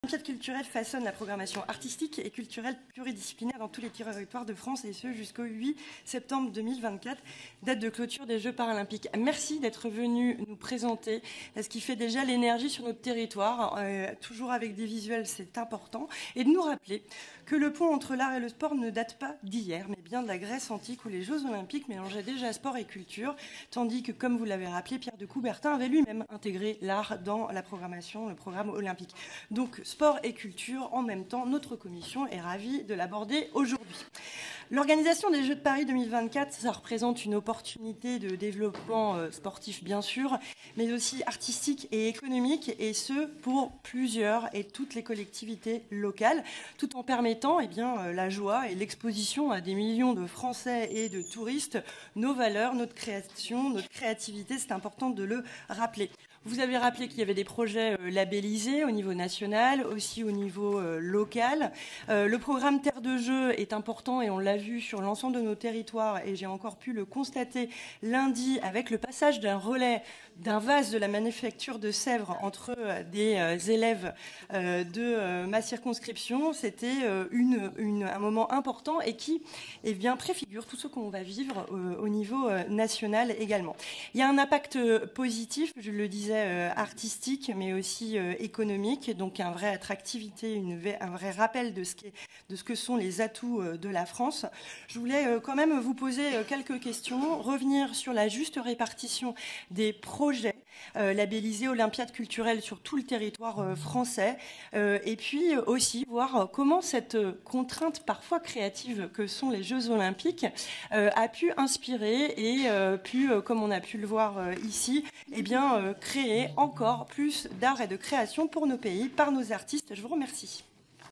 Table ronde Olympiade culturelle 17.01 - question de Céline Calvez
Table ronde sur l’Olympiade Culturelle avec Mme Dominique Hervieu, directrice en charge de la culture à Paris 2024, M. Noël Corbin, délégué général à la transmission, aux territoires et à la démocratie culturelle et M. François Laurent, délégué ministériel aux Jeux olympiques et paralympiques.

Les Jeux de @paris2024 représentent une opportunité sportive, bien sûr, mais aussi économique et artistique.

Au nom des @deputesrenaissance , je souligne l’importance de l’« Olympiade culturelle » en ce sens. Partout dans nos territoires, elle développe et fait rayonner la Culture.